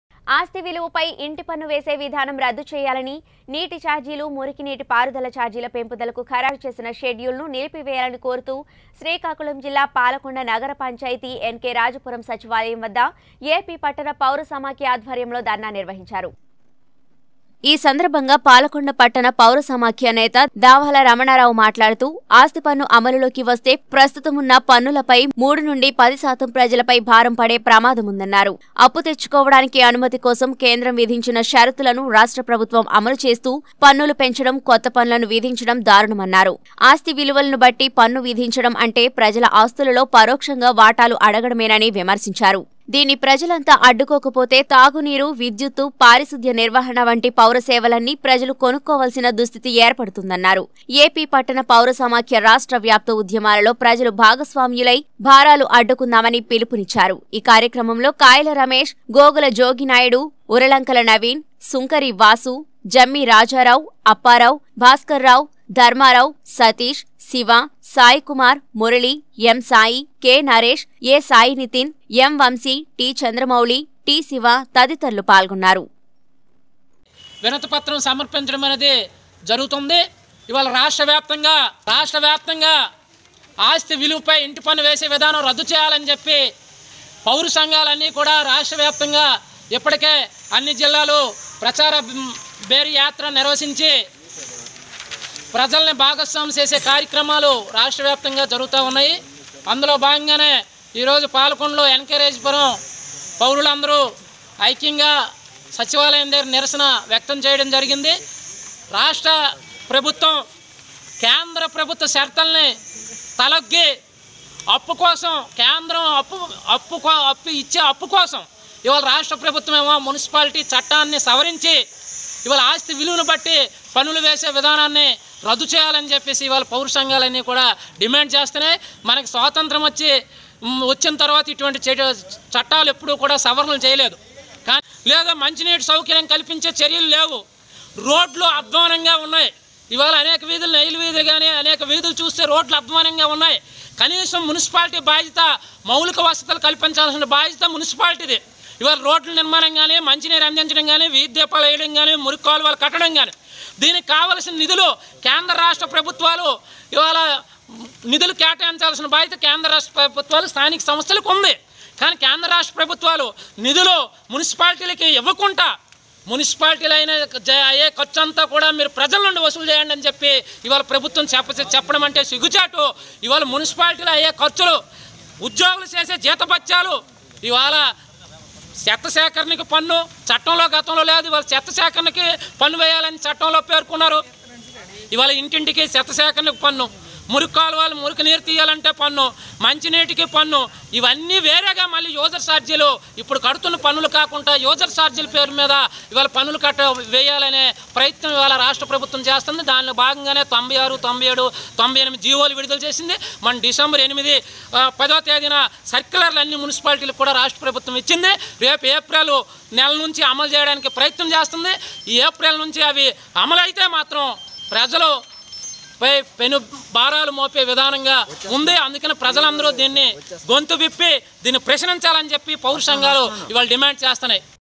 పన్నులు ఛార్జీల పెంపుదల ఖరారు చేసిన షెడ్యూల్ను నిలిపివేయాలి పాలకొండ పట్టణ పావురం సమైక్య నేతల డిమాండ్
పన్నులు ఛార్జీల పెంపుదల ఖరారు చేసిన షెడ్యూల్ను నిలిపివేయాలి పాలకొండ పట్టణ పావురం సమైక్య నేతల డిమాండ్ శ్రీకాకుళం జిల్లా | 19 Newstelugu